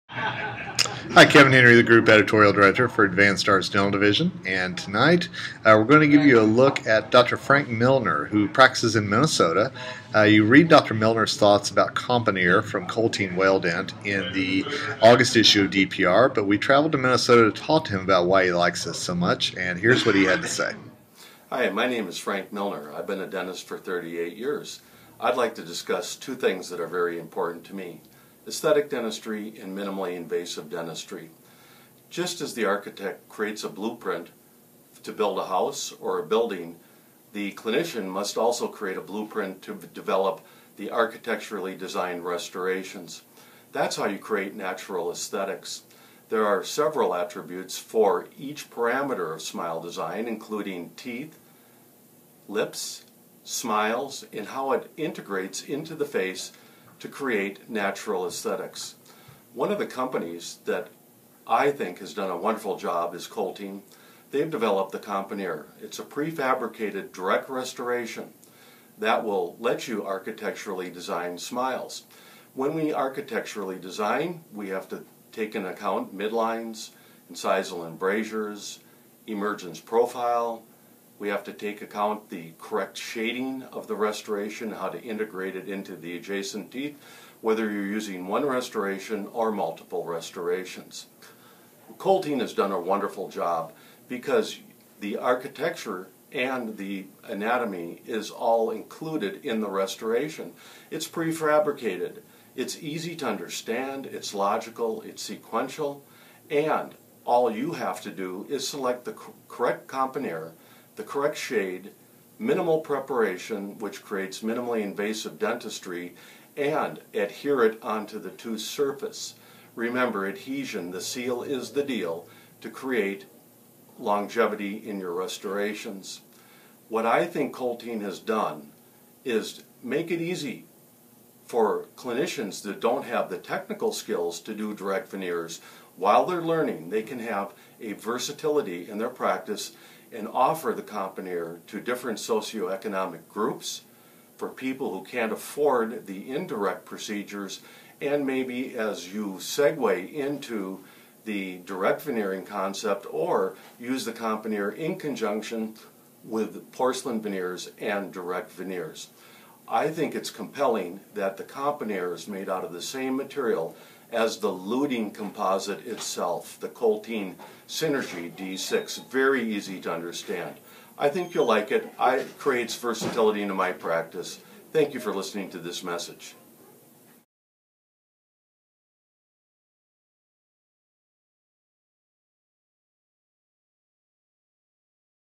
Why one dentist raves about COMPONEER from Coltene Whaledent
After briefly discussing it in the August issue of Dental Products Report, Dr. Frank Milnar of St. Paul, Minn., tells you even more about why he uses COMPONEER in his dental practice.